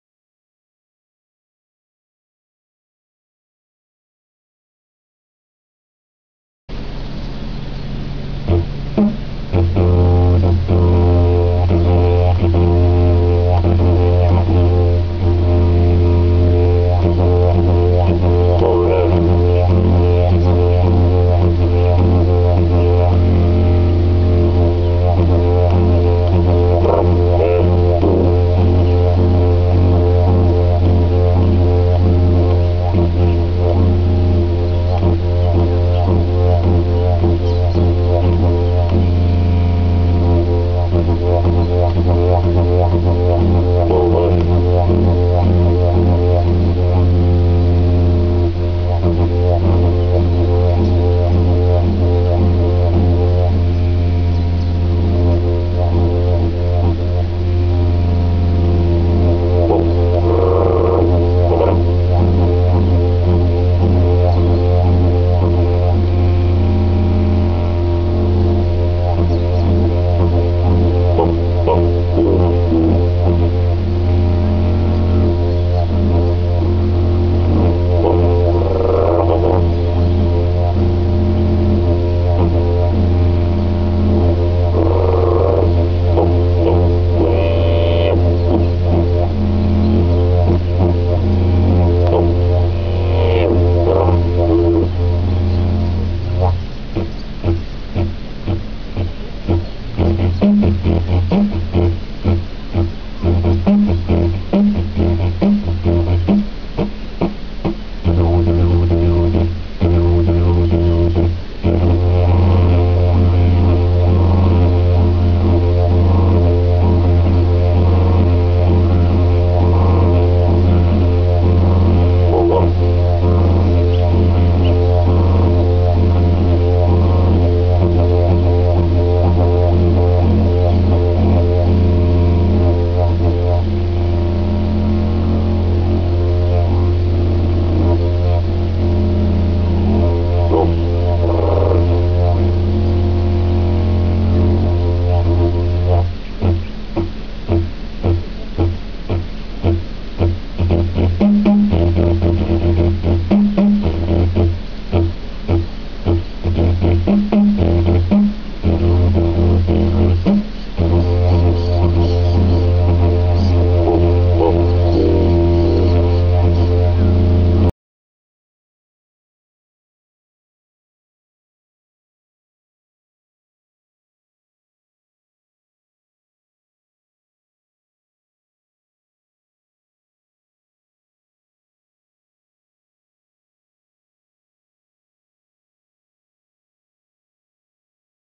Didgeridoo Pete at a Cathedral Termite Mound Australia
Didgeridoo pete playing a Colosal Sized Cathedral Termite Mound, these termite mounds are as tall underground as they are above ground!